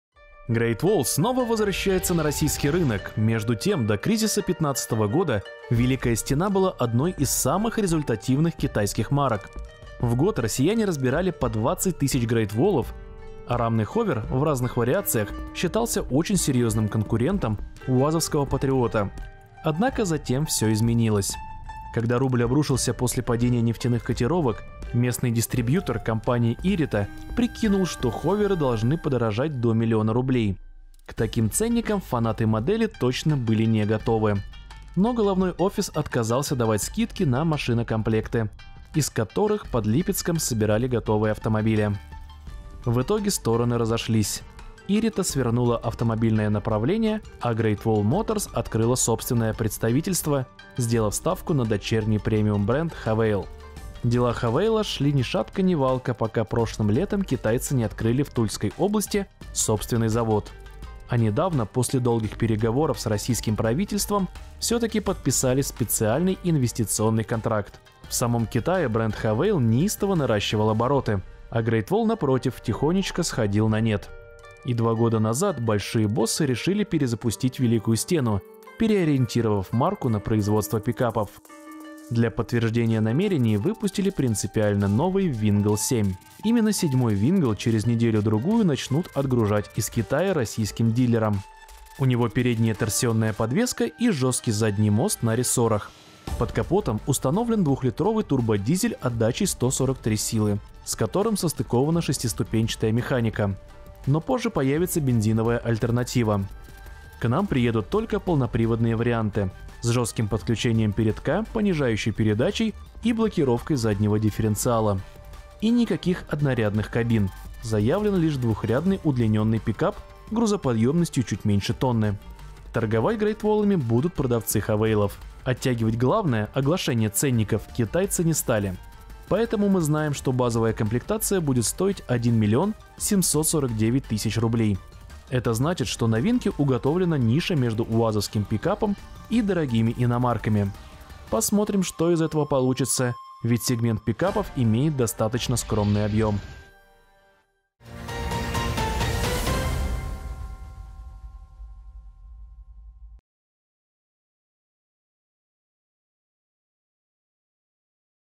Great Wall Wingle 7 восстановит «Великую стену» в России | Новости с колёс №1159
Грейт Уолл снова возвращается на российский рынок с пикапом Уингл 7. Между тем, до кризиса 2015 года «Великая стена» была одной из самых результативных китайских марок! В год россияне разбирали по 20 тысяч Грейт Уоллов, а рамный Hover в разных вариациях считался очень серьёзным конкурентом уазовского Патриота. Однако затем всё изменилось.